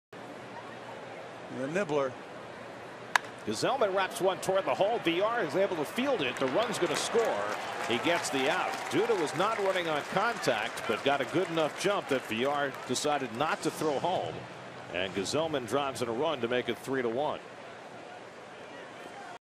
NYM@MIL: Gsellman plates Duda on RBI groundout
Robert Gsellman smacks a grounder to Jonathan Villar, who throws to first for the out while Lucas Duda scores to give the Mets a 3-1 lead